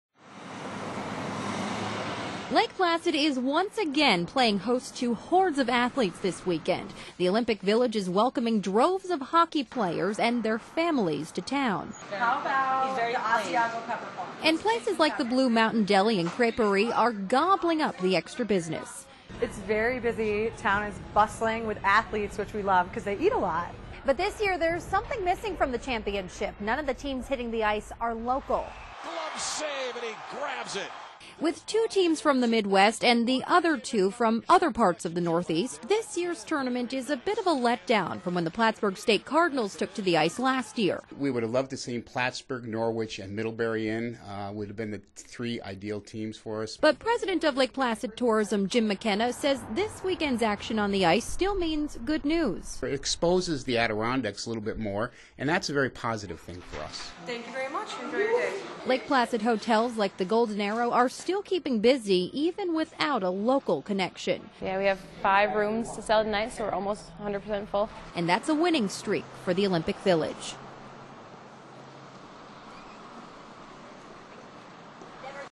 Frozen Four Economy
The frozen four Division 3 hockey championships is returning to Lake Placid. Businesses in the area say it will be a big boost to the local economy with all the fans coming to town.